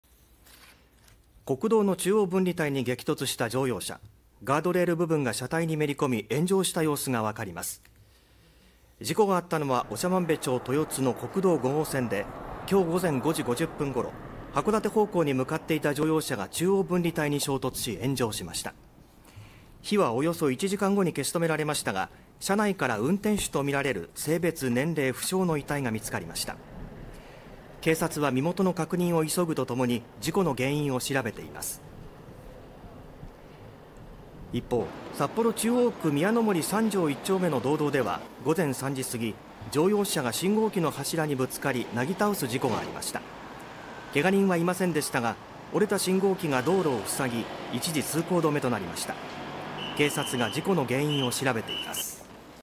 中央分離帯に激突炎上 車内から性別・年齢不明の遺体…信号機なぎ倒す事故で一時渋滞も 道内で事故相次ぐ (22/07/13 12:18)
7月13日午前、北海道道南部の長万部町で乗用車が中央分離帯に衝突して炎上する事故があり、1人が死亡しました。

　国道の中央分離帯に激突した乗用車…ガードレール部分が車体にめり込み、炎上した様子が見て取れます。

　事故があったのは長万部町豊津の国道5号線で、13日午前5時50分ごろ、函館方向に向かっていた乗用車が中央分離帯に衝突し、炎上しました。

　火は約1時間後に消し止められましたが、車内から運転手とみられる性別・年齢不詳の遺体が見つかりました。

　警察は遺体の身元の確認を急ぐとともに、事故の原因を調べています。

　一方、札幌市中央区宮の森3条1丁目の道道では、13日午前3時過ぎ、乗用車が信号機の柱にぶつかり、なぎ倒す事故がありました。

　けが人はいませんでしたが、折れた信号機が道路を塞ぎ、一時通行止めとなりました。

　警察が事故の原因を調べています。